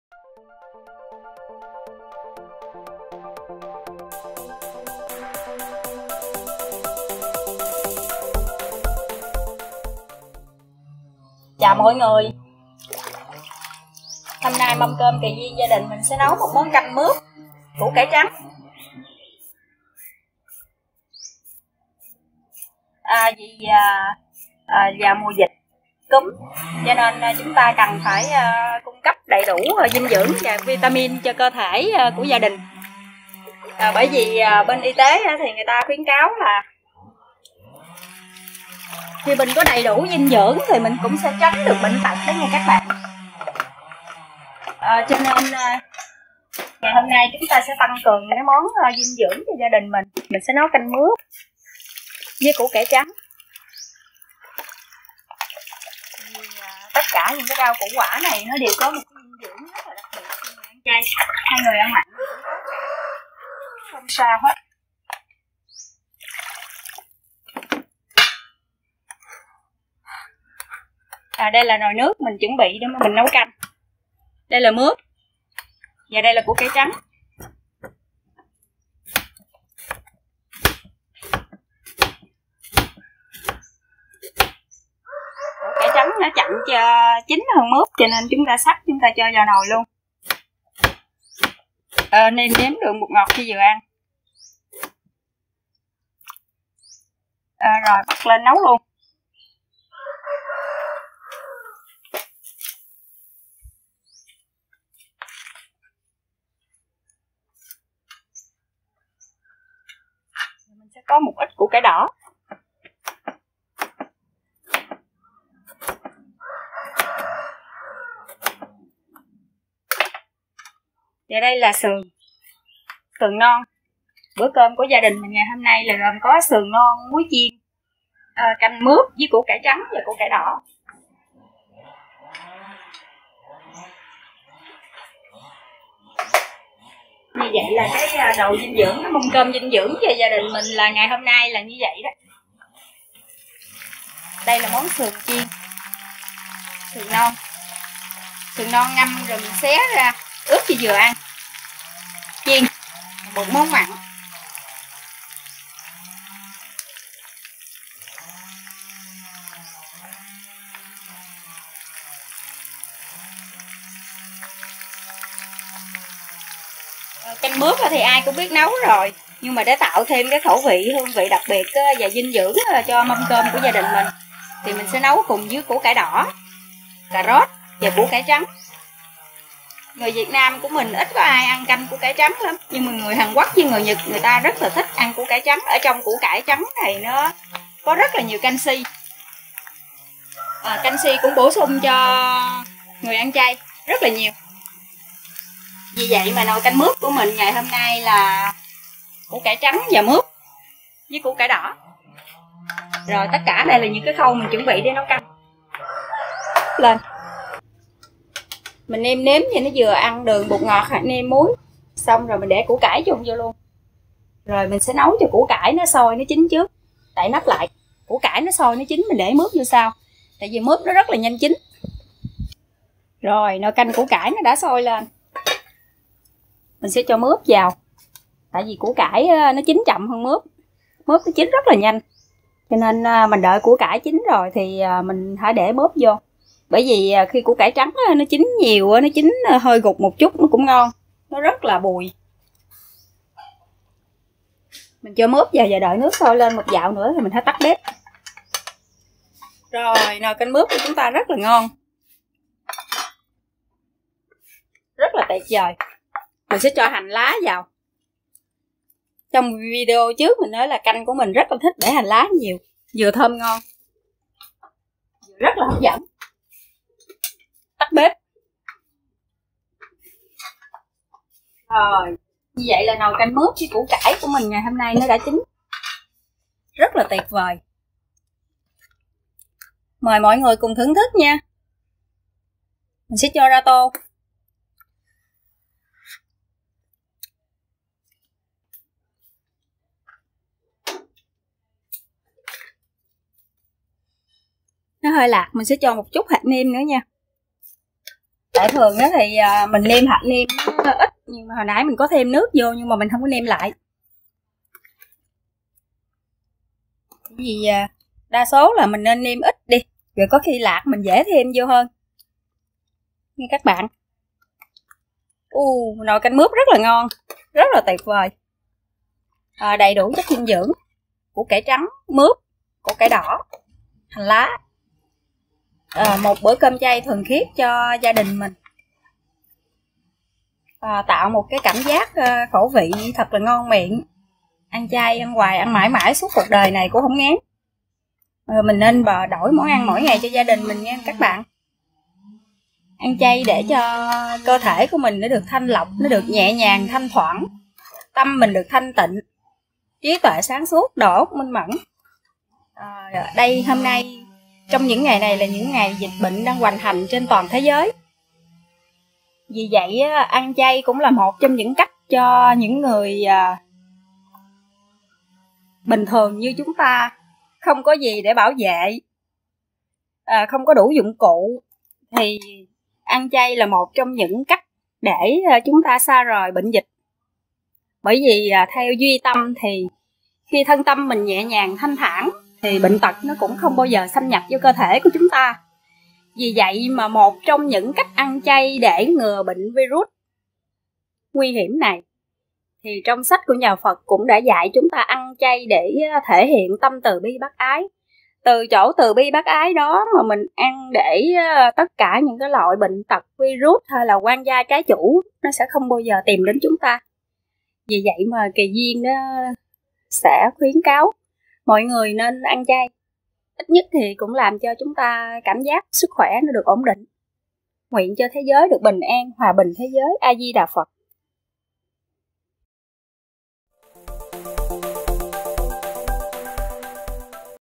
Kỳ Duyên Ngộ Quán Ăn Chay - Canh Mướp
Học nấu canh mướp với Kỳ Duyên. Phạm vi công cộng.